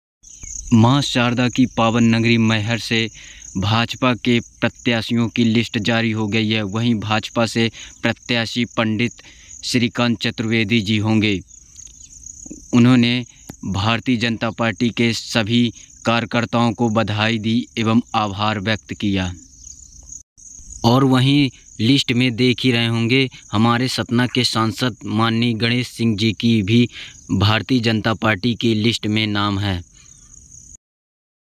मैहर मां शारदा की पावन से भाजपा विधानसभा प्रत्याशी बने श्रीकांत चतुर्वेदी जी भाजपा का आभार
मैहर मां शारदा की पावन से भाजपा विधानसभा प्रत्याशी बने श्रीकांत चतुर्वेदी जी भाजपा का आभार#cm#pm#politics pmmodi cm बीजेपी मैहर श्रीकांत चतुर्वेदी जी भाजपा विधान सभा प्रत्याशी मैहर#